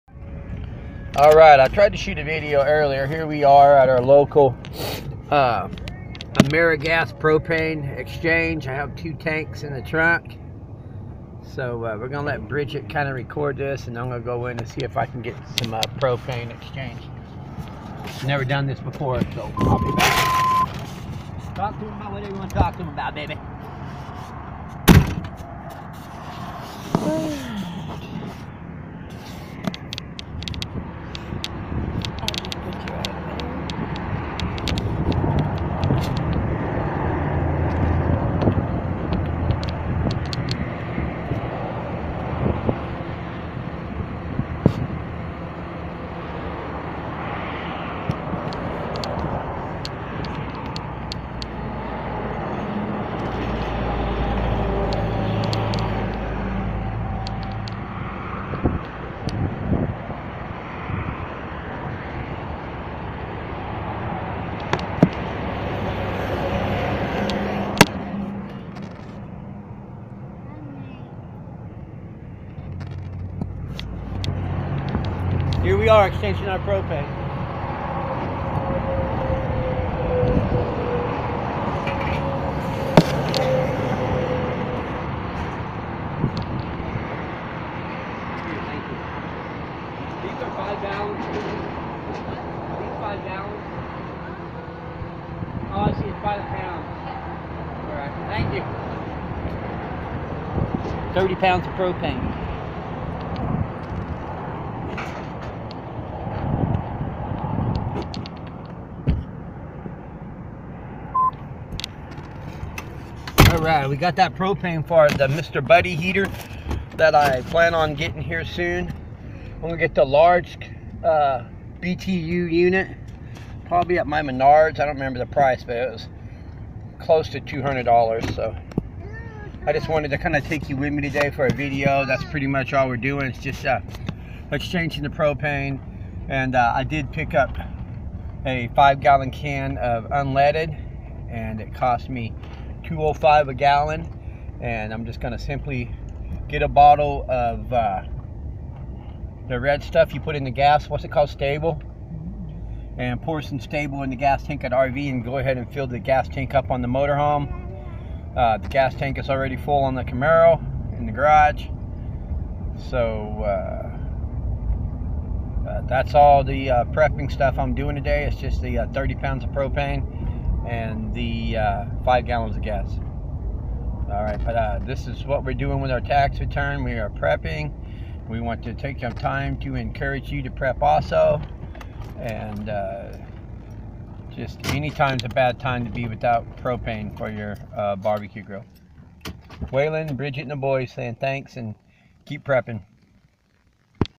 Propane exchange
Its so easy and smart to get rid of your old tanks for these with new valves and fresh paint